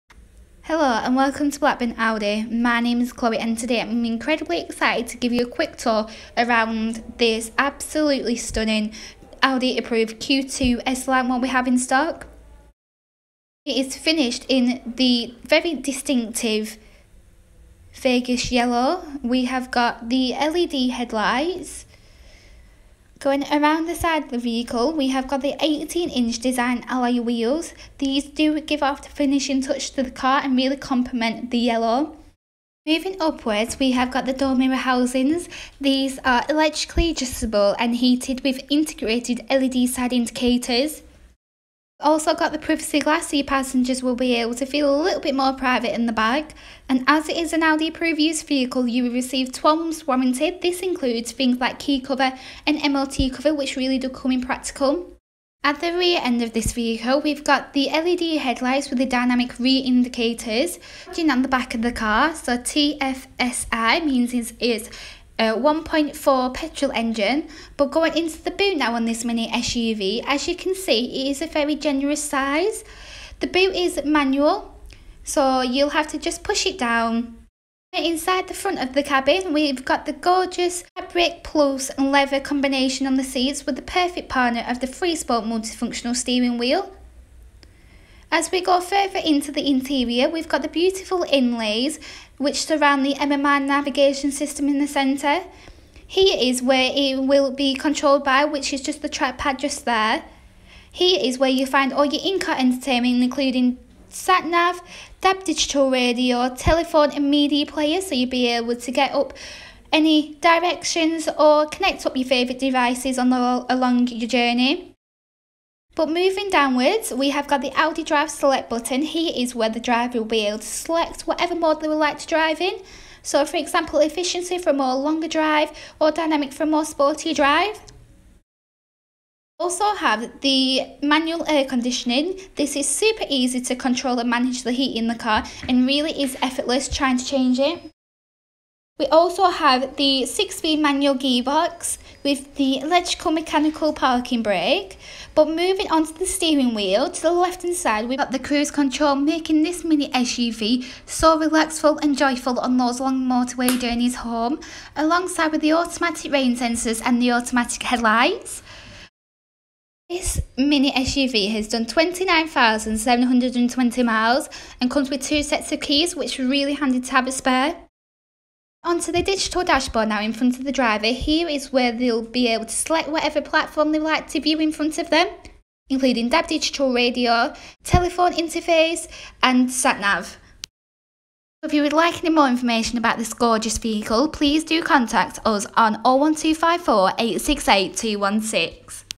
Audi Approved Q2 S-Line 1.4 Petrol Manual | Blackburn Audi
Just into our stock, this Audi Q2 in Vegas Yellow, is now available for sale from Blackburn Audi.

Reg number: PY66 NUA
Fuel: Petrol
Transmission: Manual
Engine Size: 1.4
Mileage: 29,725 miles

Features include:
Fabric "Pulse"/leather combination
Illuminated door sill trims with S logo
Dark-tinted windows (privacy glass)
18in Alloy Wheels - 5-Spoke Y Design
Cruise control
Audi Drive Select
Audi Pre-Sense Front with Pedestrian Recognition